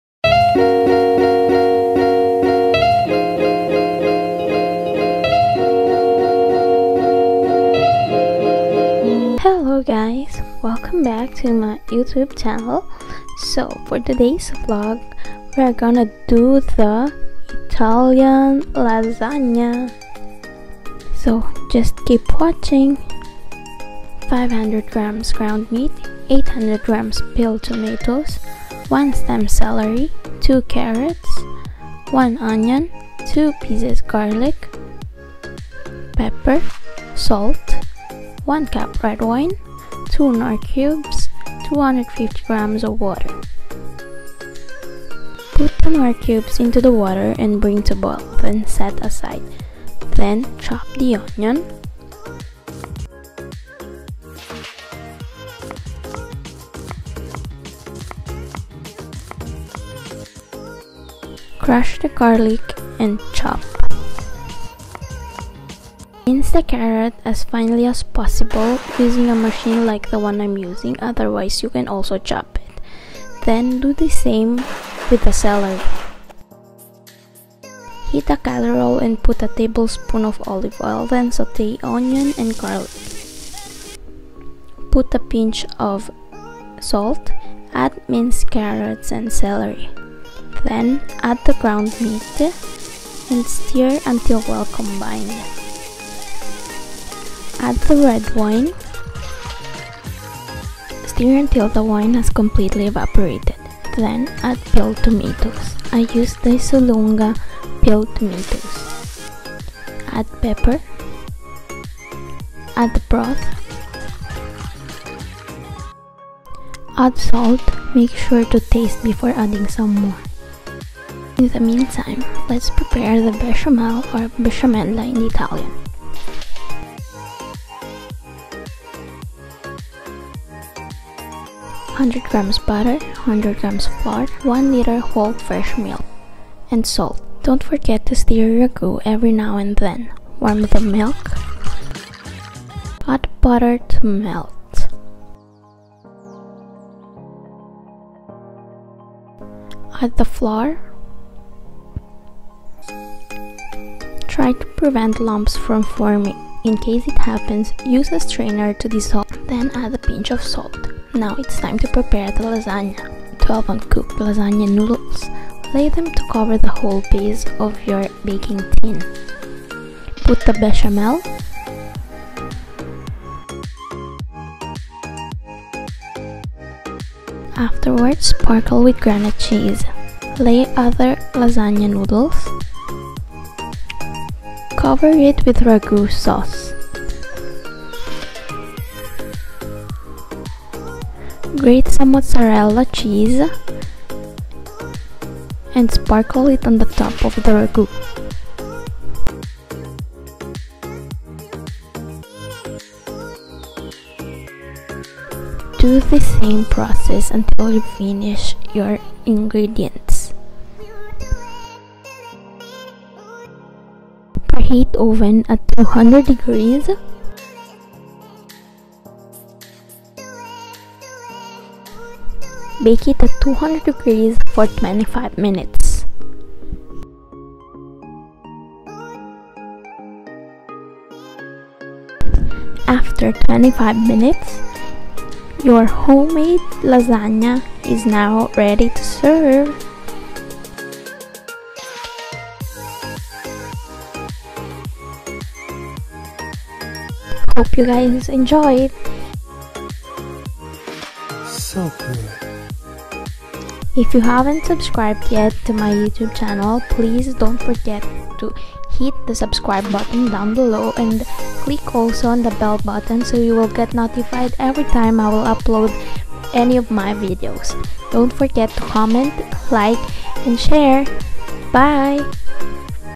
Italian Lasagna Recipe | Home made Bechamel & Ragù Sauce | Leyvlogs
Hellopeople! For today's vlog i'm gonna show you how to cook Italian Lasagna from scratch. Meaning we're gonna do from Bechamel, Ragù and finally the most awaited Lasagna.

So keep on watching! hope you like it.